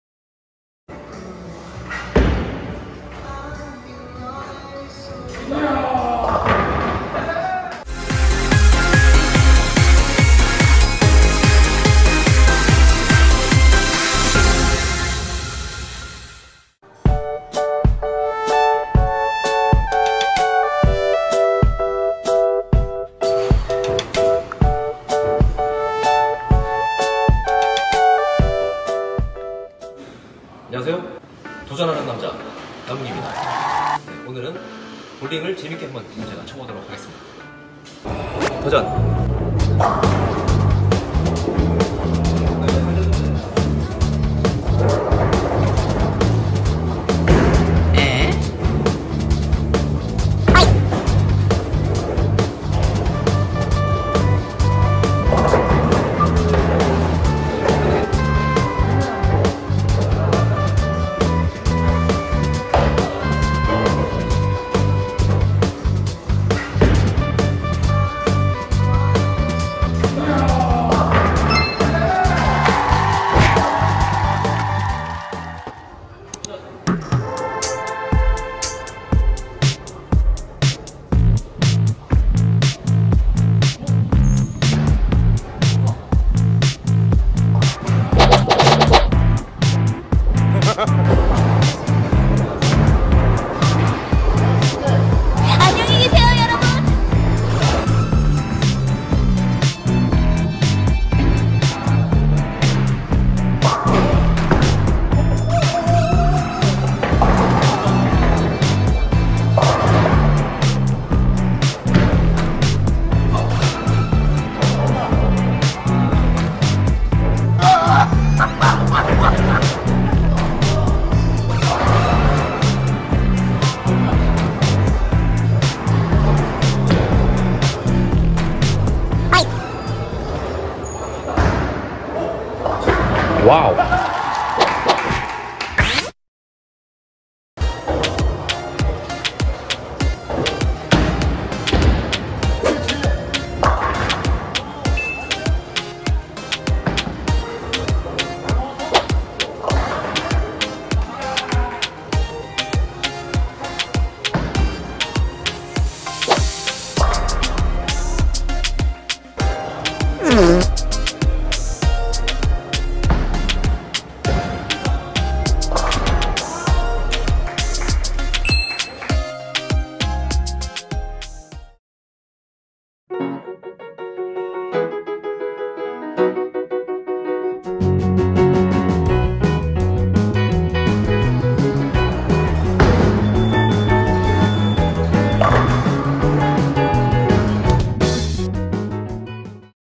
양손 볼링 도전!
재미있게 볼링 한 게임 쳤습니다 ㅎㅎ
여러분도 한번?  도전 해보세요~ 😄

✅ 볼링장 사장님께 협조하에 촬영하였습니다~

 Bowling is fun to play like this🔮
you try it! 😄

• Classic bowling center